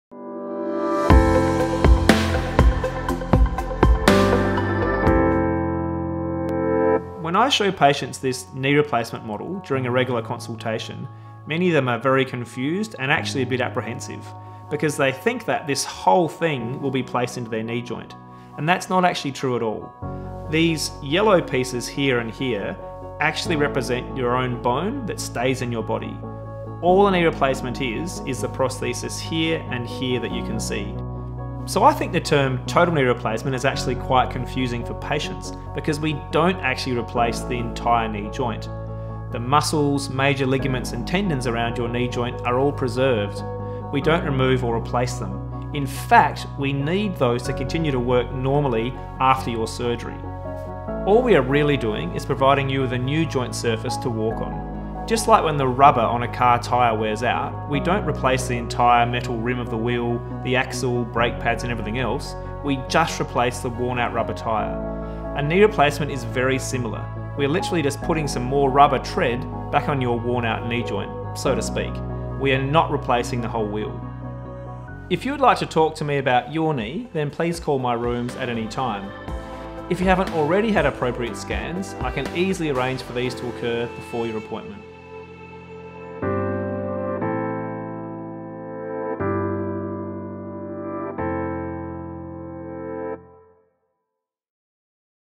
What is a knee replacement?
Does a knee replacement actually replace your entire joint? You might be surprised by the answer... Find out everything you need to know in this educational video.

Dr Matthew Hutchinson is an orthopaedic surgeon practicing in Adelaide, South Australia. He has a special interest in knee replacement using 3D patient-specific technology.